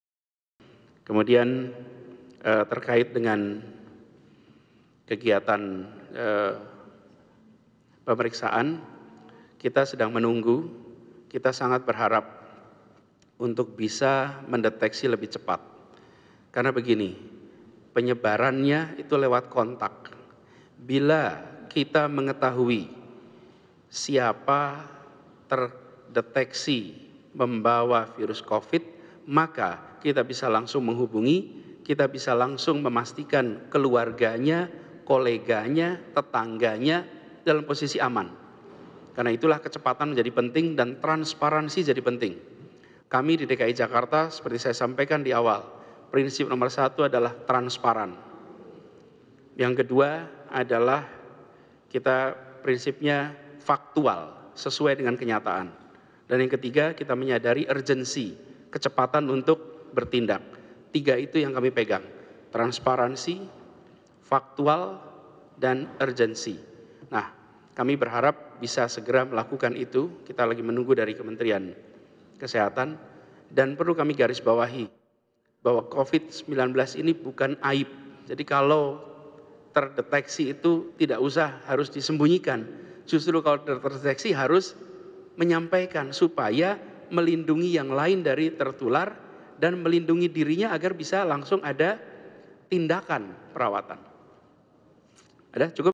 Bukan Sebuah Aib, Anies Minta Warga Transparan Soal Corona
JAKARTA, KOMPAS.TV - Hari ini (14/03) di Kantor Balai Kota, Gubernur DKI Jakarta Anies Baswedan memberikan himbauan kepada seluruh masyarakat terkait maraknya penyebaran virus corona yang membuat sebagian warga resah.

Anies menegaskan untuk setiap orang harus berani benar-benar transparan dalam menyampaikan hal-hal yang dirasa ada gejala yang mendekati adanya virus corona tersebut.

\"Bila kita mengetahui siapa terdeteksi membawa Covid-19, maka kita bisa langsung menghubungi, kita bisa langsung memastikan keluarganya, koleganya, tetangganya dalam posisi aman. Karena itulah kecepatan menjadi penting dan transparansi menjadi penting.\" kata Anies Baswedan kepada media usai melaksanakan rapim.

Anies juga menyampaikan ada tiga hal yang menjadi prinsip penting yang harus dipegang Pemprov DKI dalam mengatasi virus corona diantaranya adalah transparan, faktual dan urgensi.

Anies juga menambahkan bahwa Covid-19 ini bukanlah suatu aib.

\"Dan perlu kami garis bawahi bahwa Covid-19 ini bukan aib, jadi kalau terdeteksi itu tidak usah harus disembunyikan, supaya melindungi yang lain dari tertular dan melindungi dirinya agar bisa langsung ada tindakan.\" tegas Anies.

Sebelumnya, pemprov DKI memutuskan untuk menutup sementara seluruh sekolah selama dua pekan dimulai pada besok Senin (16/03/2020).

\"Pemprov DKI Jakarta memutuskan untuk menutup semua sekolah di lingkungan Provinsi DKI Jakarta dan akan melakukan proses belajar mengajar melalui metode jarak jauh dan bagi peserta Ujian Nasional yang akan berlangsung hari Senin (16/03/2020) besok serta Ujian Sekolah diputuskan juga ditunda.\" kata Anies Baswedan.

Penutupan sekolah ini berlaku selama dua minggu dan akan dilakukan review kembali di pekan kedua untuk melihat perkembangannya.